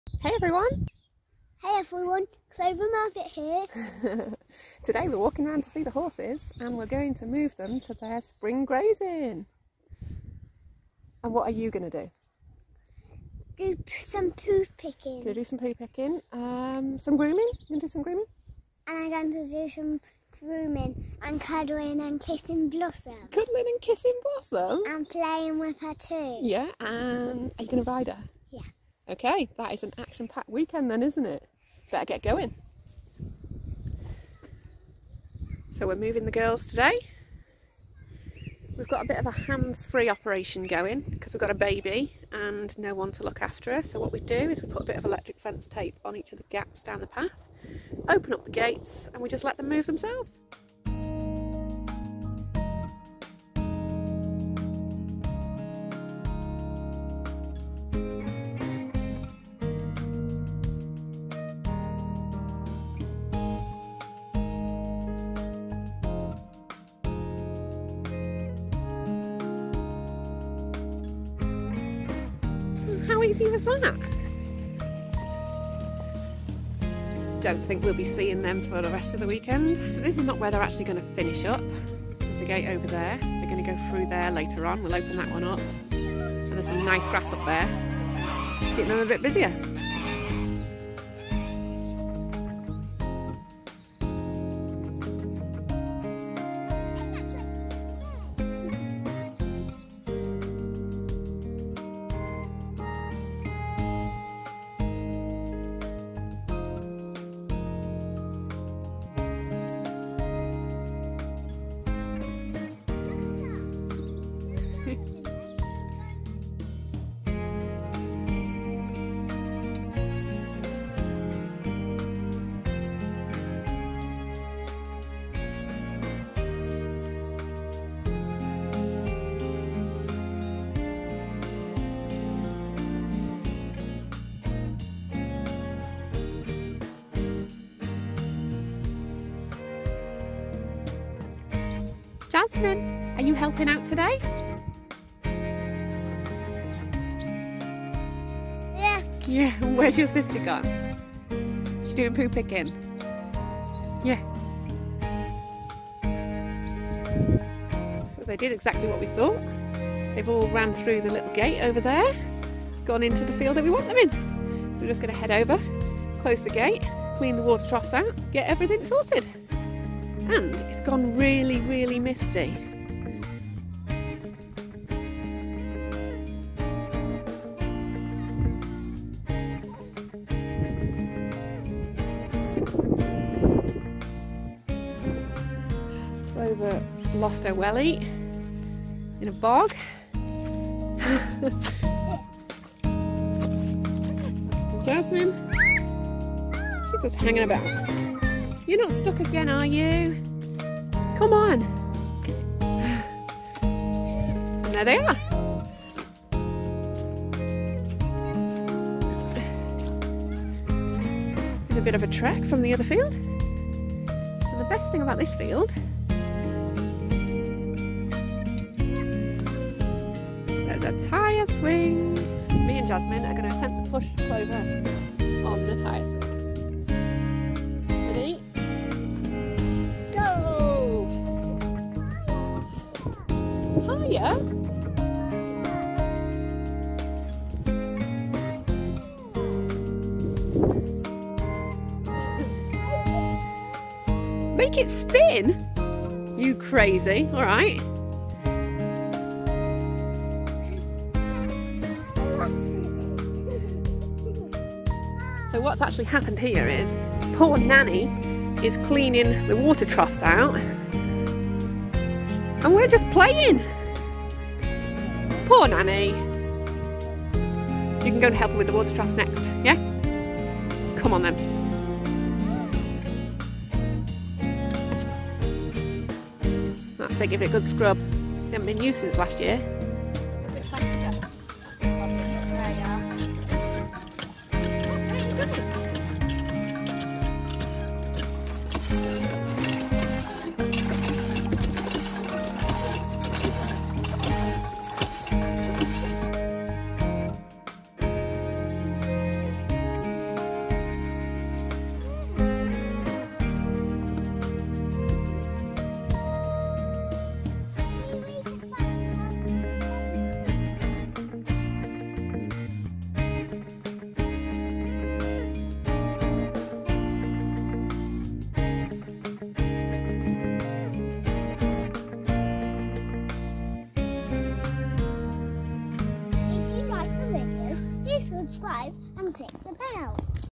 Pony Weekend Vlog - Moving The Horses Barn Vlog | Pony Fun
Today we moved the horses and ponies from their winter grazing to their spring grazing. We have a small baby and only 2 adults to move 4 horses so we opt for a hands free approach. We fence off the path, open the gates and let the horses move them selves. We were helped today by 4 year old Clover who helps to open the gates, clean the water troughs, fill them up and cuddle the ponies and of course test the tyre swing in the new field.

Horses moved are:
Ex racehorse (Libby)
Coloured Cob (Spicy)
11hh Pony (Daisy)
Mini Shetland Pony (Blossom)